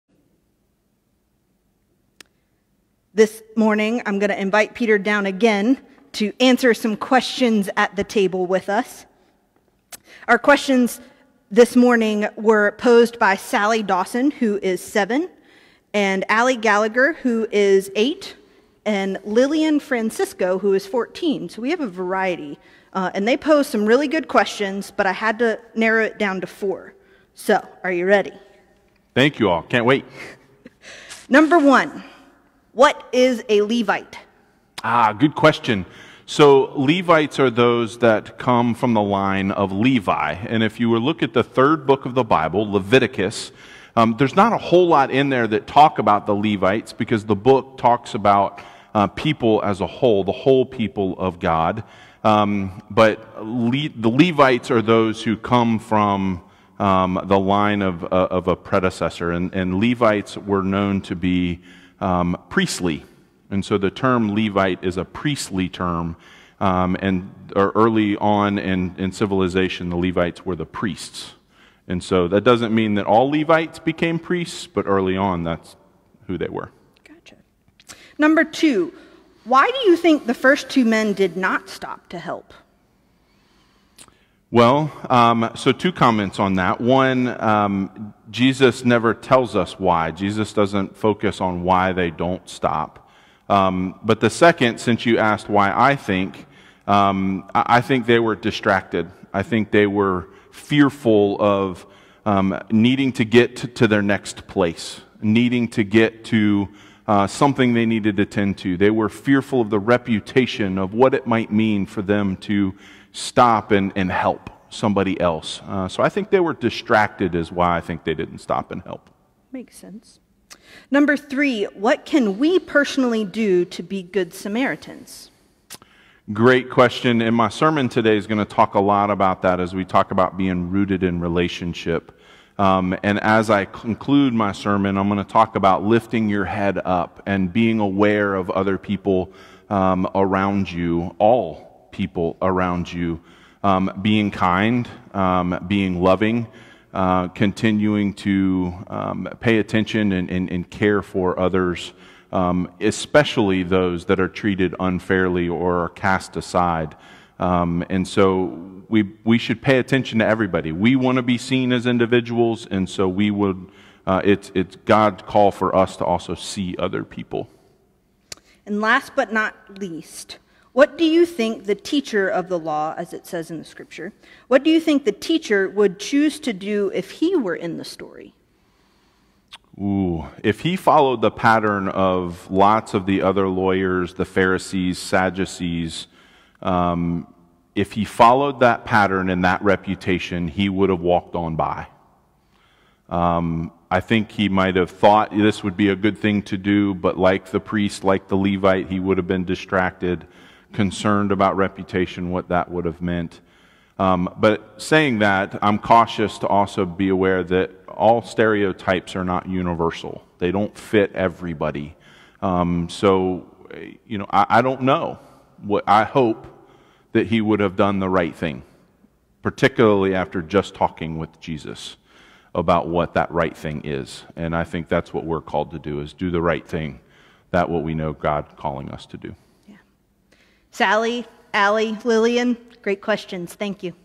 Questions At the Table--Good Samaritan
The last couple of weeks, our virtual worship has included "Questions at the Table." Each week, our children and youth submit a few questions they have from the scripture texts for that Sunday. While standing at the table, Kathleen fires the questions at our pastors who have to think quick on their feet and share their answers live.